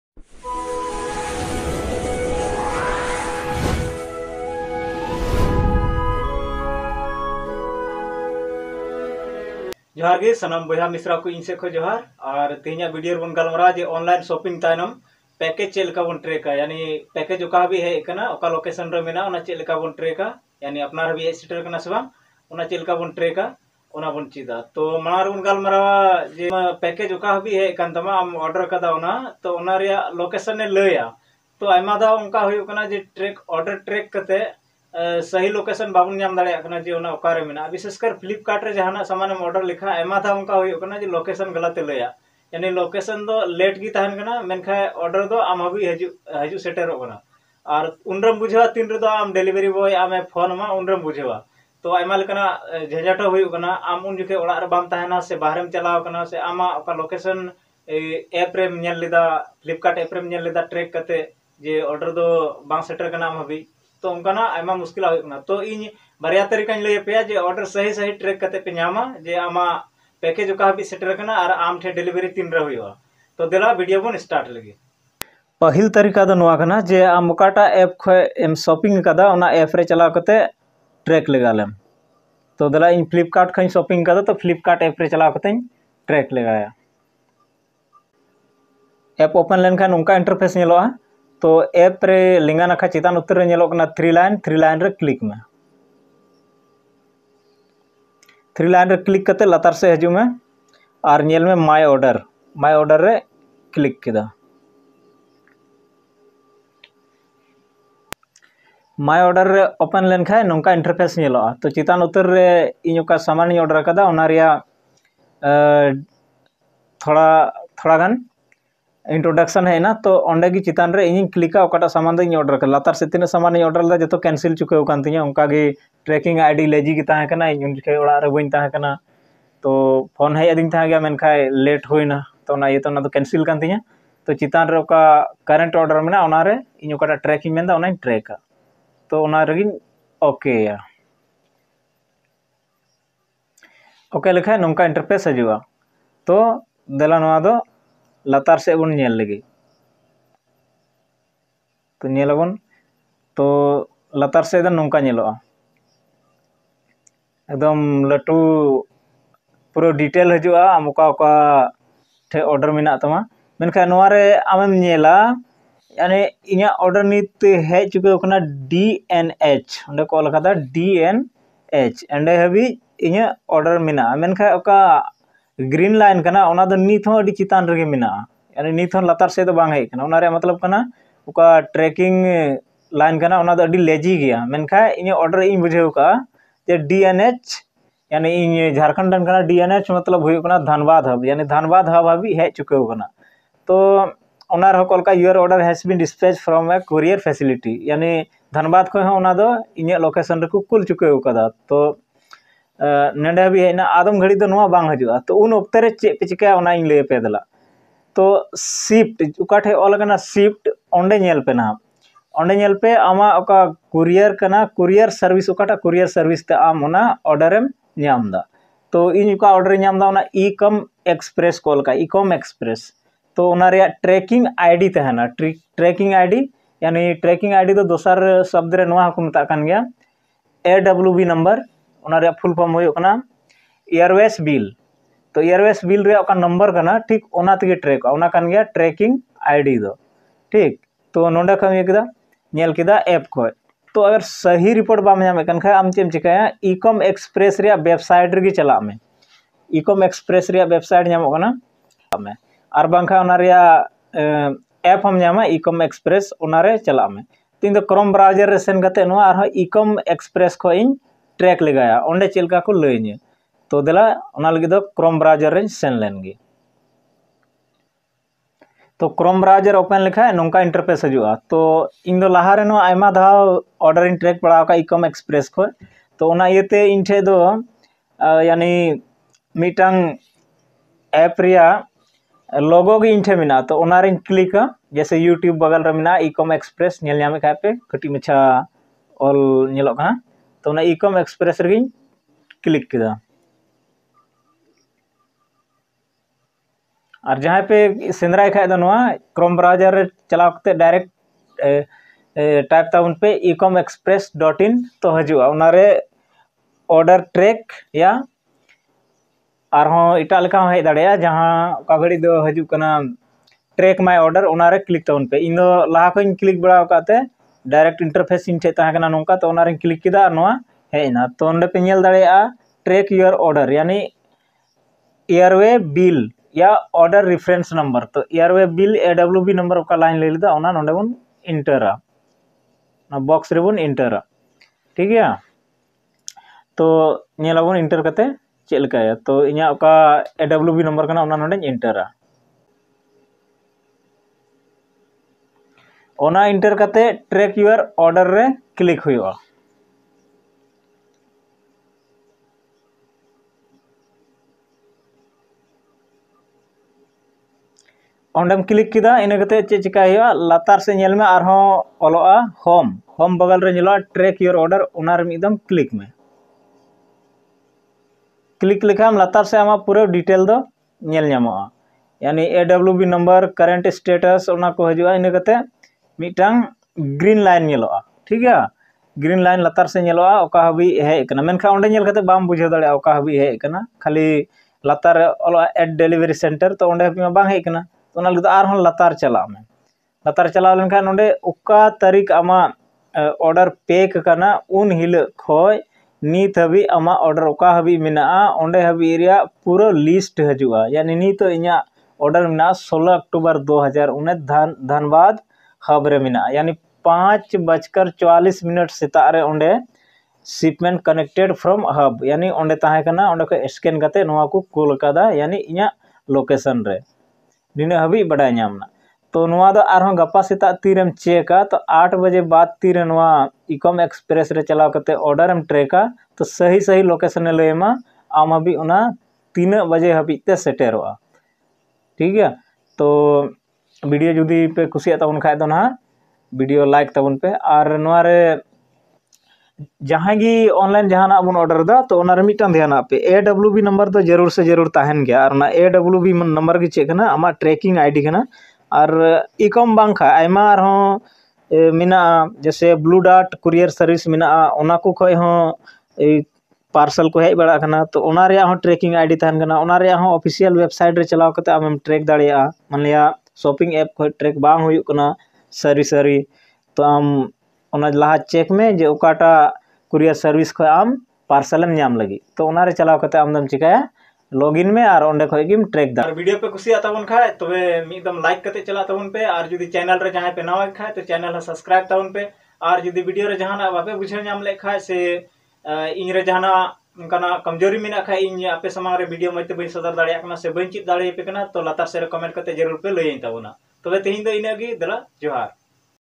How to track Flipkart order!! in santali#hembrom tech
Nowa video re Flipkart order track reyak' barya tarika layakan.nowa video Nel katek cet'leka pe bujhaw keda Ona do comment katek layanpe.judi video moj pe bujhaw ket' Khan channel ho subscribe kak'pe.Cedak se hembrom tech do hamesha nonkan videos kuy aguyeda.